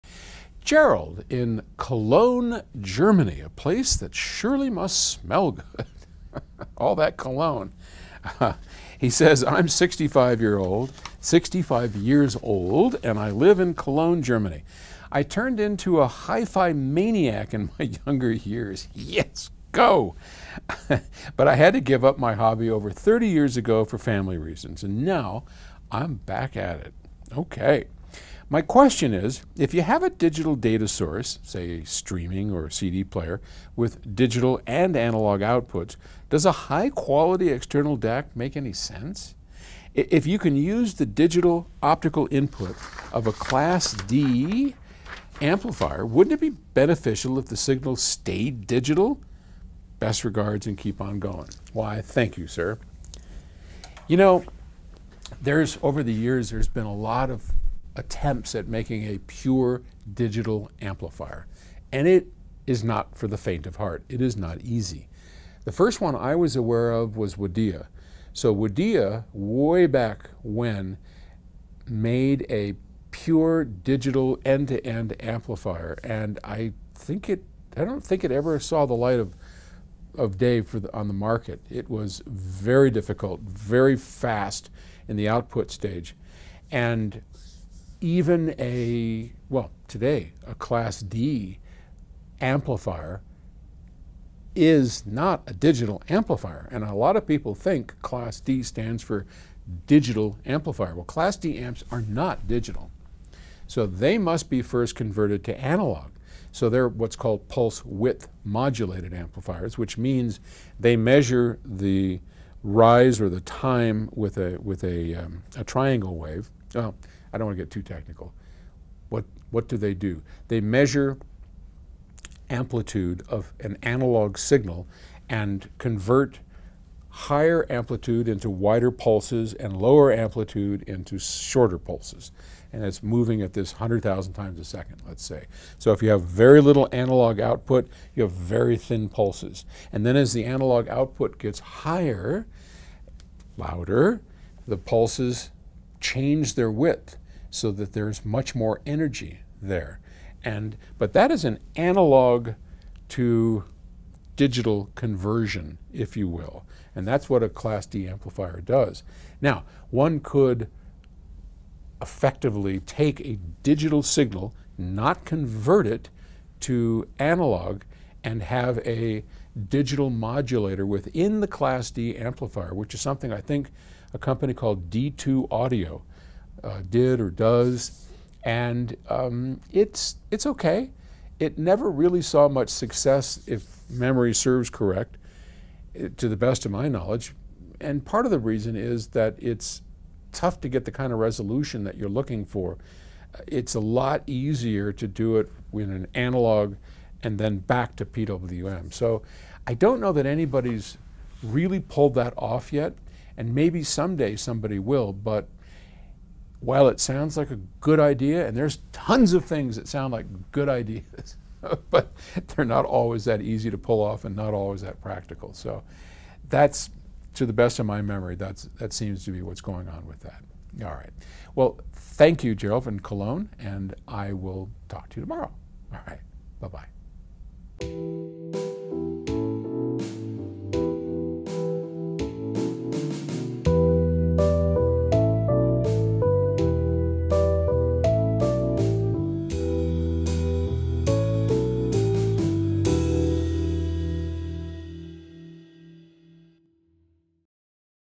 A pure digital amplifier
Why do Class D amplifiers require conversion to analog before they work? Find out the secrets behind digital amplification.